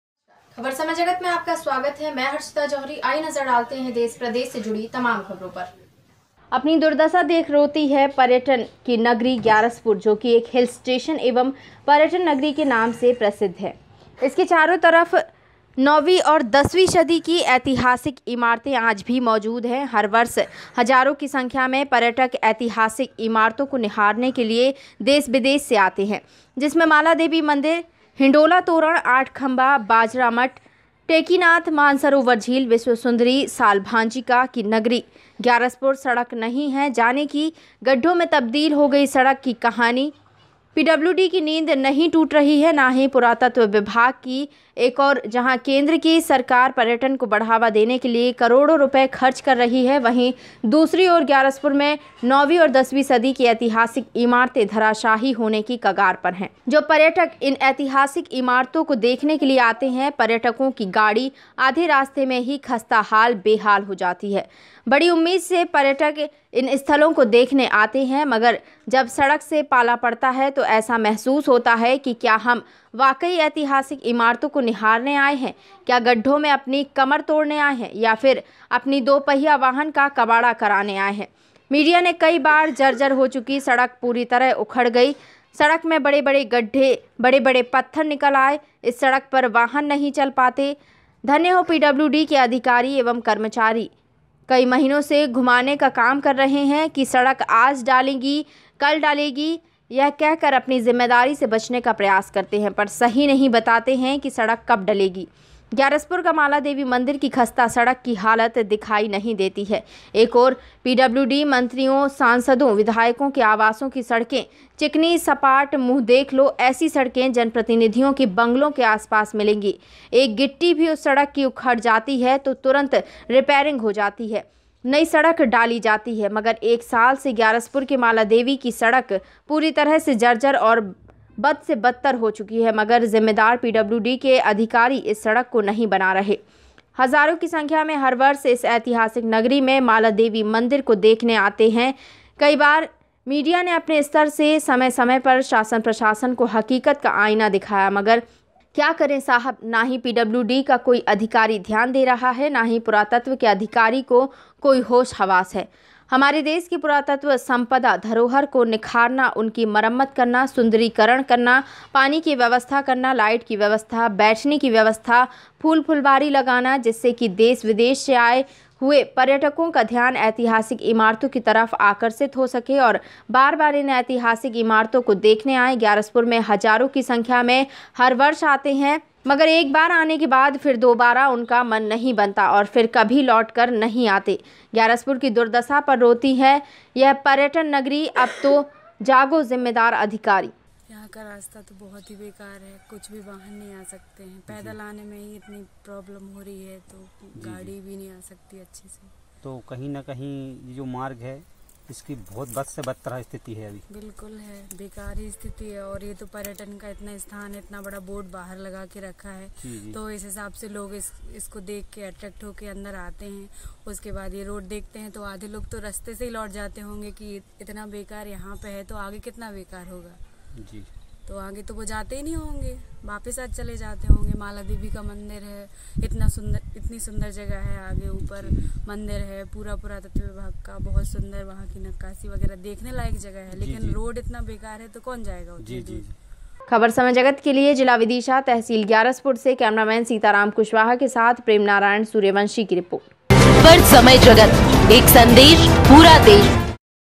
अपनी दुर्दशा देख रोती है यह पर्यटन की नगरी
ग्यारसपुर से प्रेम नारायण सूर्यवंशी की रिपोर्ट